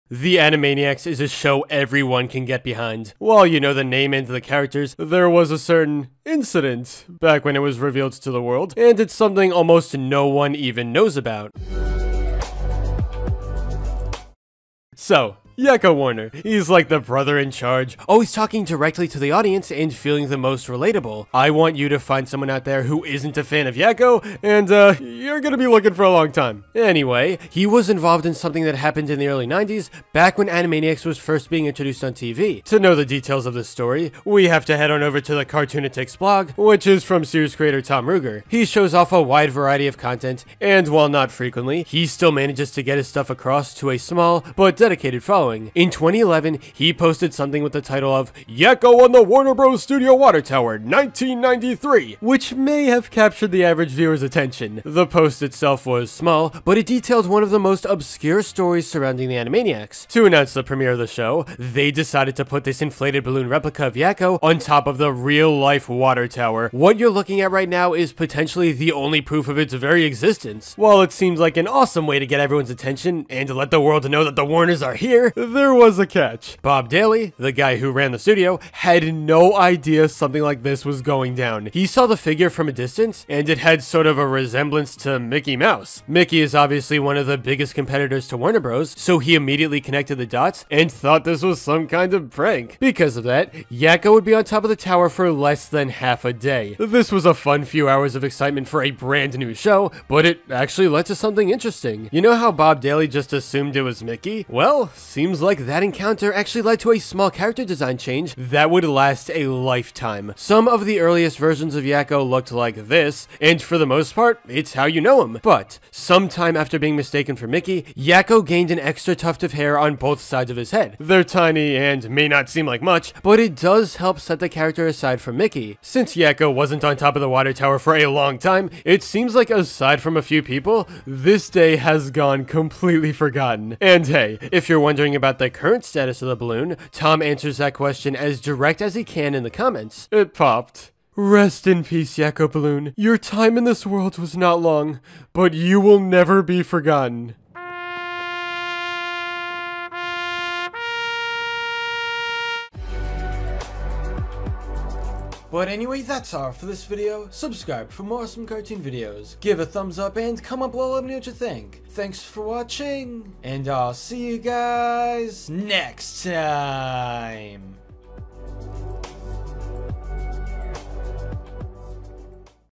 Why Did Yakko Warner Become Real? (Animaniacs 1993)
The new Animaniacs revival is coming back for Season 2, but there's a story surrounding the ORIGINAL Animaniacs that no one even knows about. What happened on the day that Yakko Warner entered real life atop the Warner Bros tower? Animaniacs creator Tom Ruegger tells us!

✭ SOCIAL MEDIA ✭